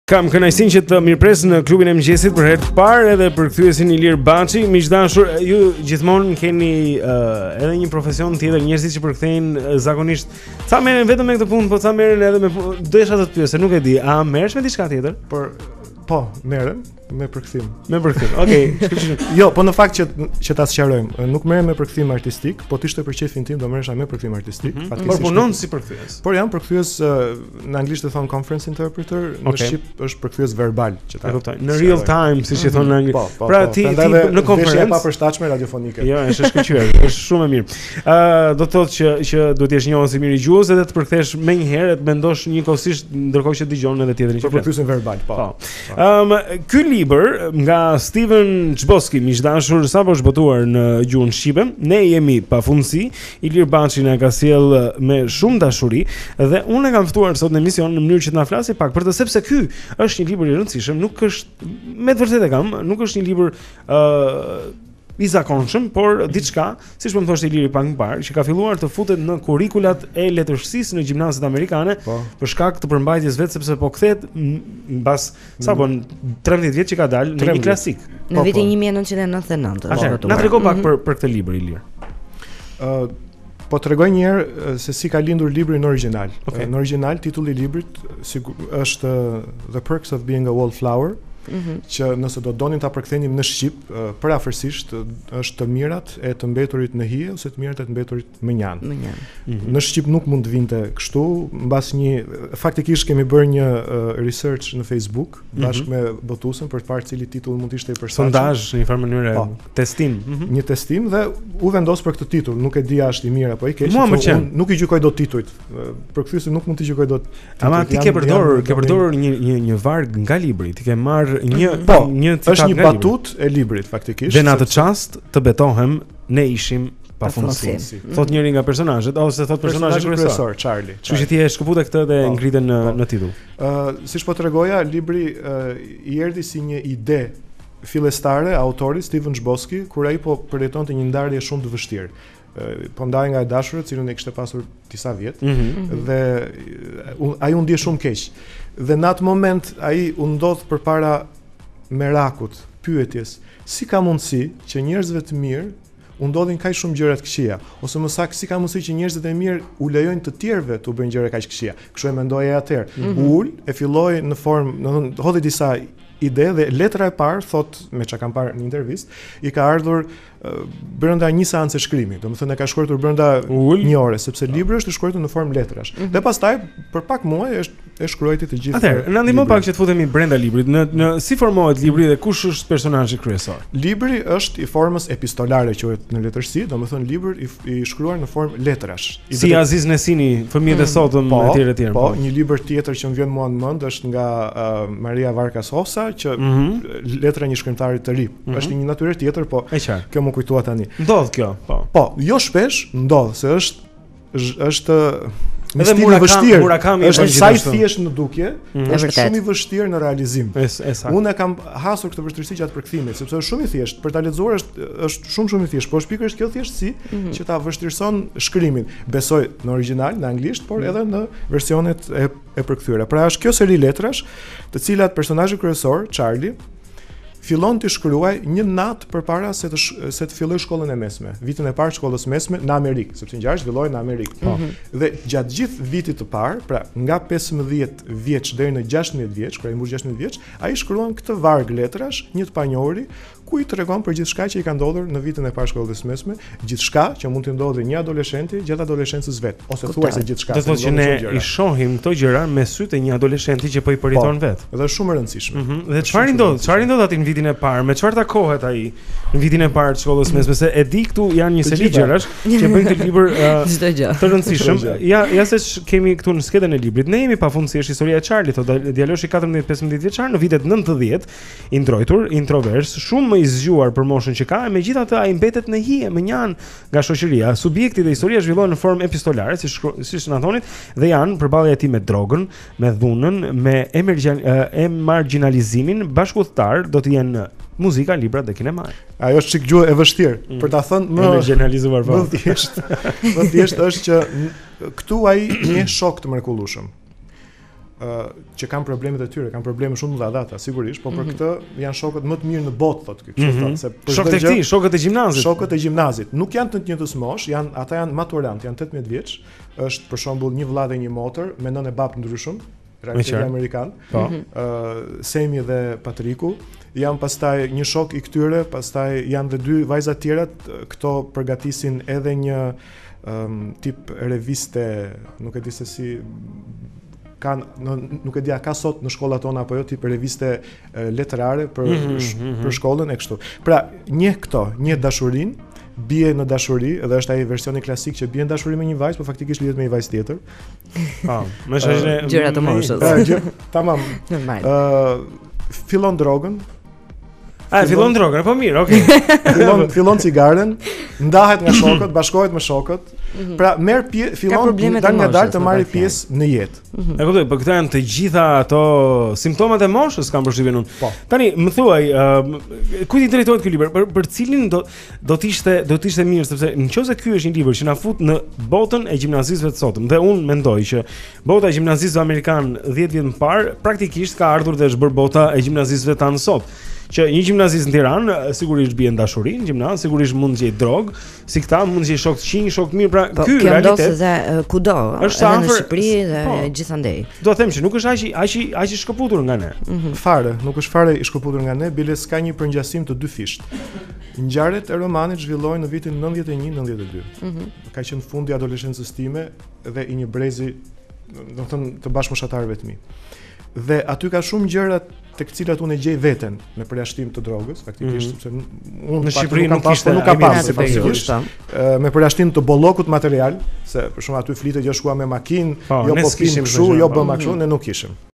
Perkthyesi Ilir Baçi ne Klubin e mengjesit - Pjesa e Pare
Flasim per librin, "Ne jemi pafundesi" (The Perks of Being a Wallflower) nag Stephen Chbosky.